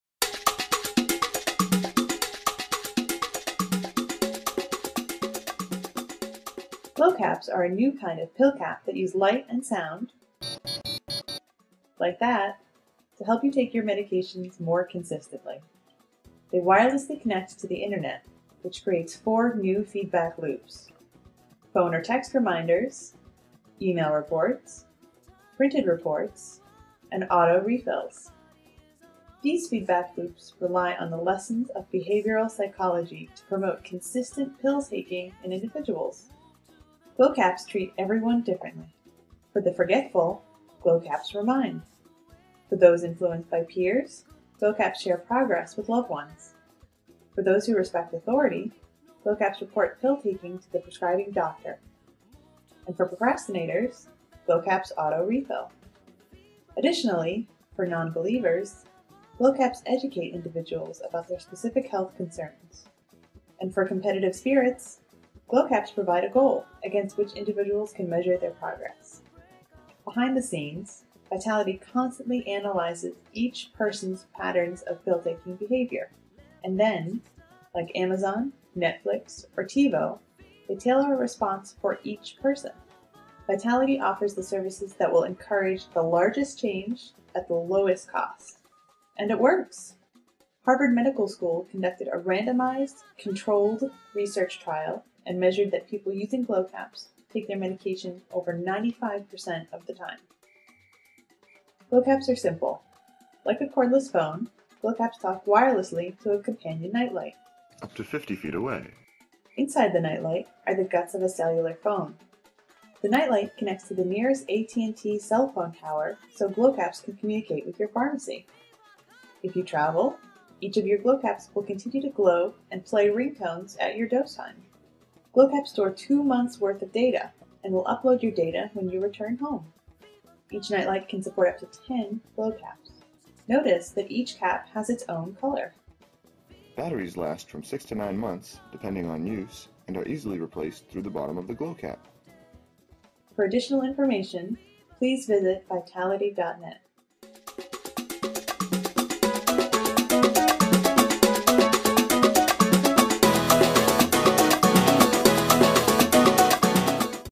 GlowCaps, FAQs
This video explains some of the finer details of Vitality's GlowCaps. Topics include: what GlowCaps are, how GlowCaps work, how well they work, additional offered services, connectivity, travel ability, and data storage.